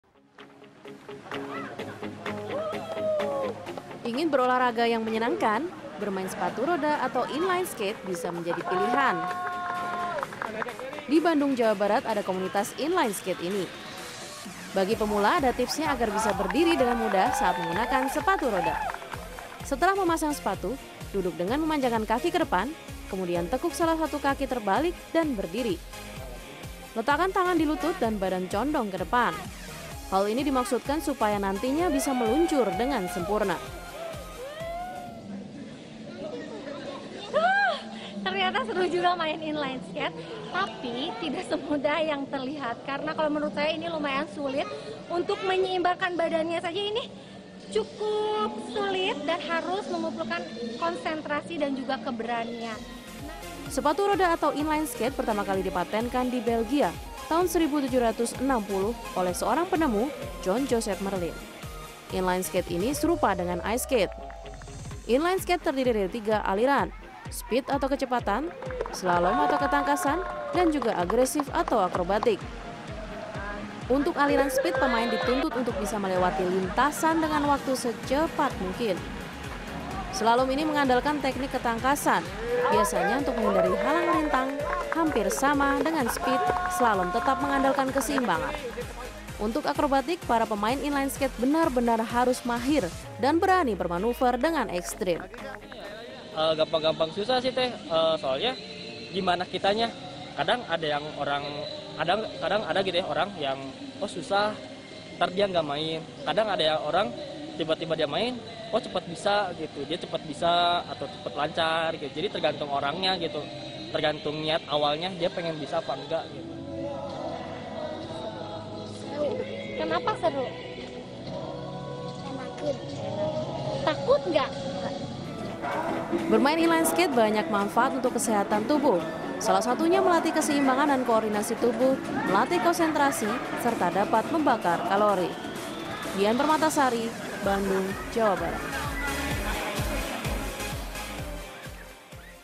Sensasi Meluncur Dengan Inline Skate
Gaya hidup sehat bisa dimulai dengan beragam pilihan olahraga, salah satunya adalah bermain sepatu roda.

Selain membuat tubuh lebih bugar, kegiatan ini memberi banyak manfaat bagi kesehatan.

Like & Follow Facebook:
CNN Indonesia

cnnindonesiatv